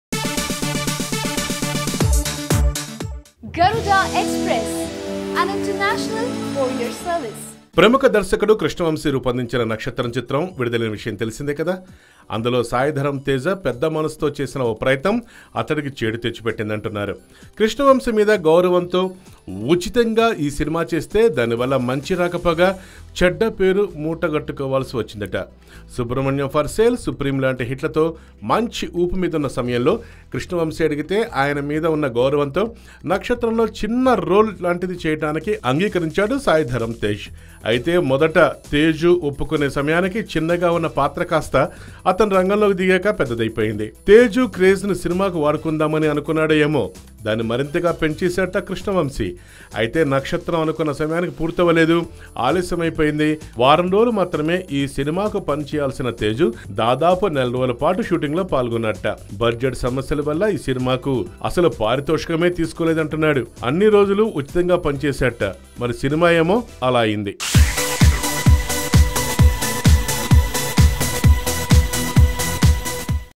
మంచి తనంతో చెడ్డ పేరు వచ్చింది | Sai Dharam Tej Reveles About Nakshatram Movie | YOYO Cine Talkies
Sai Dharam Tej Reveles About Nakshatram Movie.
మంచితనంతో చెడ్డపేరు వచ్చింది.